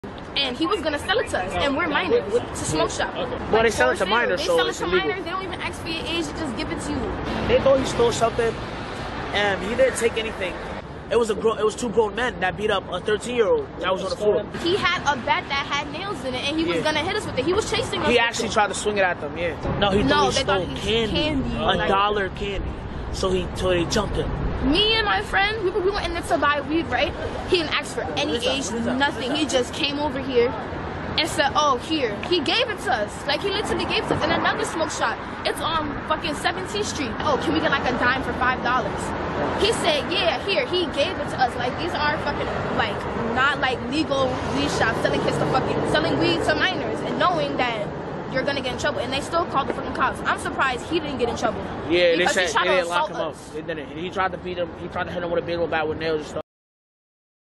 Teens Put Smoke Shop On Blast For Selling Them Weed
Just hours before the body footage was captured on July 25 last year, Mark Hecko, 26, stabbed Adrian Ellingford, 44, twice.

Officers arrived at the scene shortly after the assault to find the victim with wounds so severe that a bone had been broken in his chest.

However, Hecko astonishingly returned to the crime scene to taunt the police.